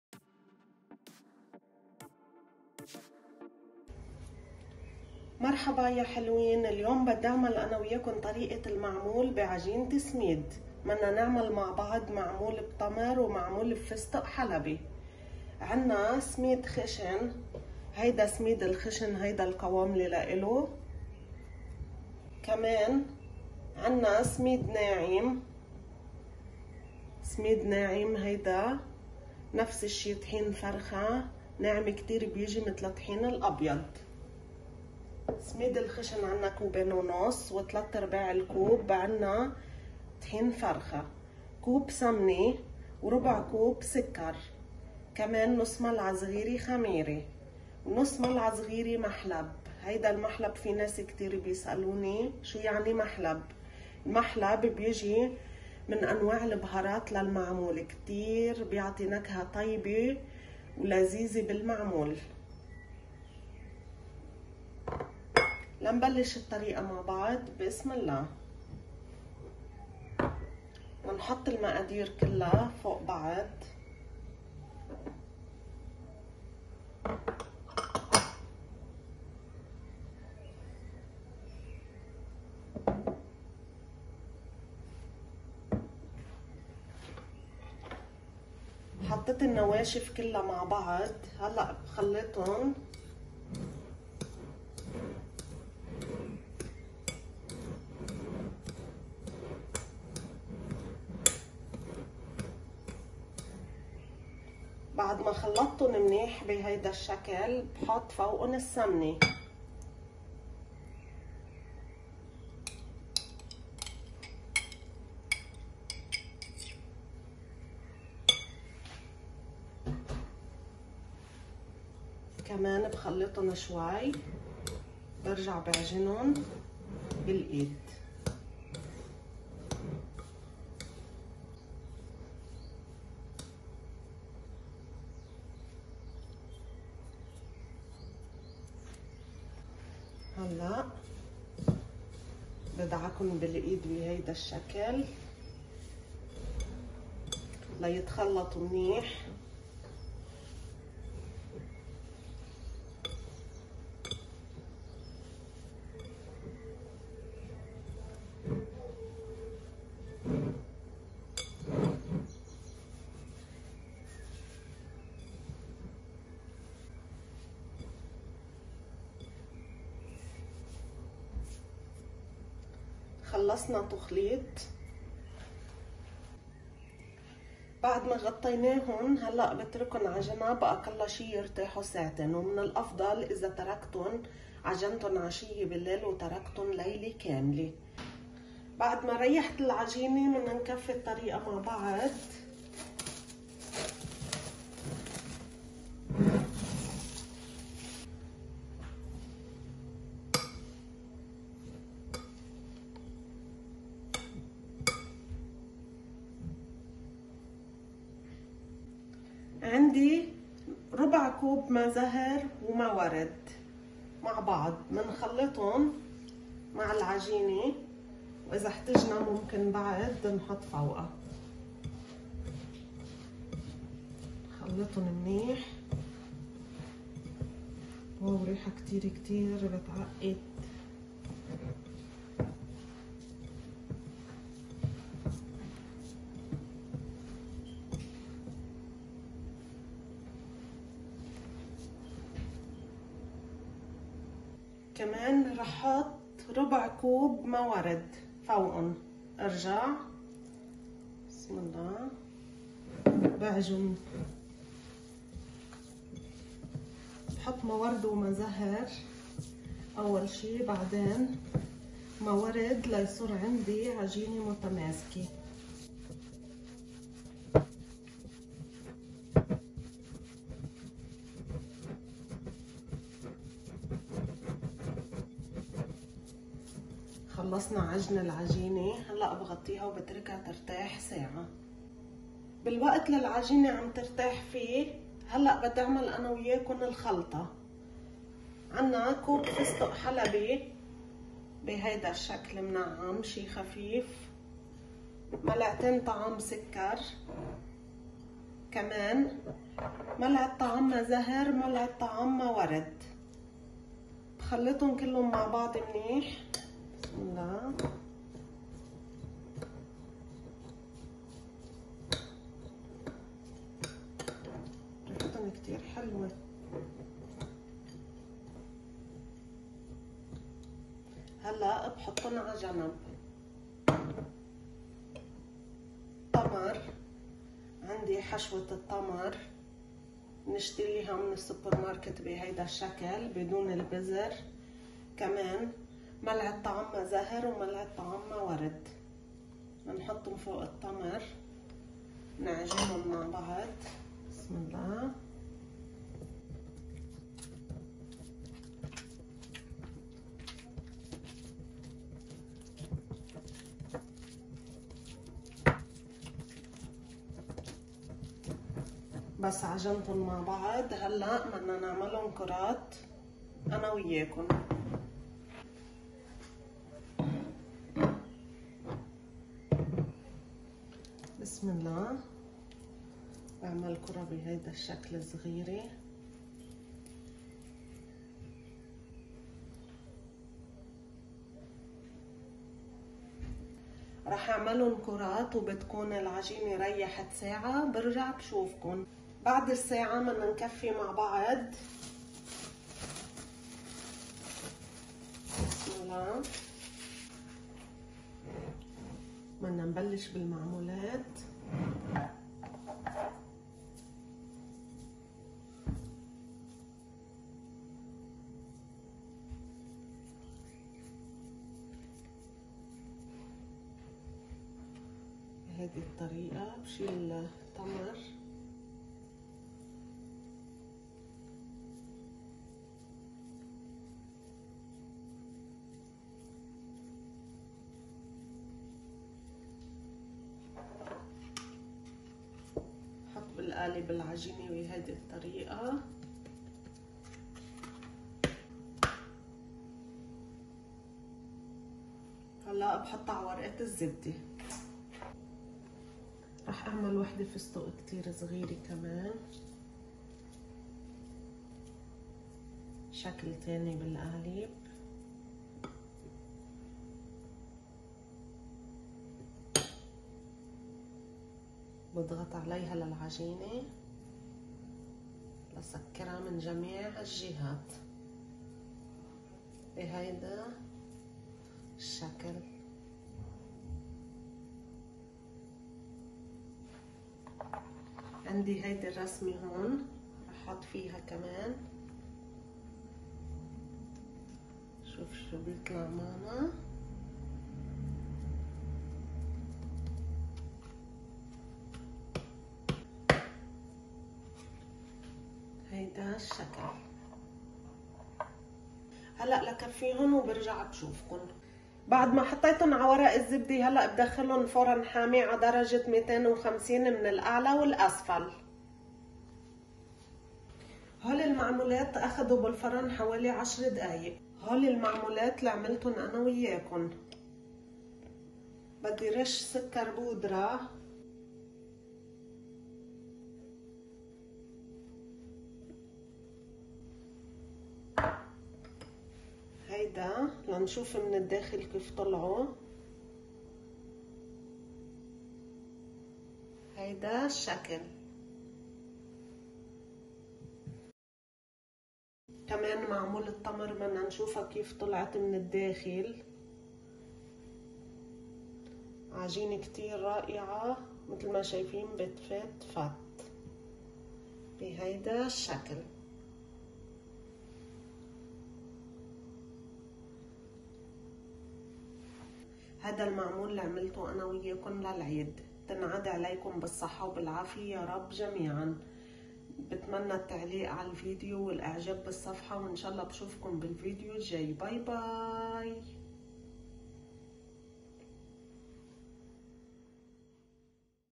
Semolina Dates & Pistachio Cookies - أسهل طريقة لمعمول السميد بالفستق الحلبي والتمر- العجوة
المقادير
٢و ١/٢ سميد خشن
٣/٤ سميد ناعم
١ كوب سمنة
١/٤ كوب سكر
١/٢ ملعقة صغيرة خميرة
١/٢ ملعقة صغيرة محلب
١/٤ كوب ماء زهر وماء ورد
١/٤ كوب ماء ورد
الحشو
١/٢ كيلو تمر عجوة
١ ملعقة طعام ماء زهر
١ ملعقة طعام ماء ورد

١/٢ كوب فستق
٢ ملعقة طعام سكر
١ ملعقة طعام ماء زهر
١ ملعقة طعام ماء ورد
بتمنى التعليق على الفيديو والإعجاب بالصفحة
Ingredients
2 1/2 cup of semolina
3/4 cup of fine semolina
1 cup of pure butter ghee
1/4 cup of sugar
1/2 teaspoon of yeast
1/2 teaspoon of mahaleb
1/4 cup of blossom water & rose water
1/4 cup of rose water
1/2 KG dates
1 tablespoon of blossom water
1 tablespoon of rose water
1/2 cup of pistachio
2 tablespoons of sugar
1 tablespoon of blossom water
1 tablespoon of rose water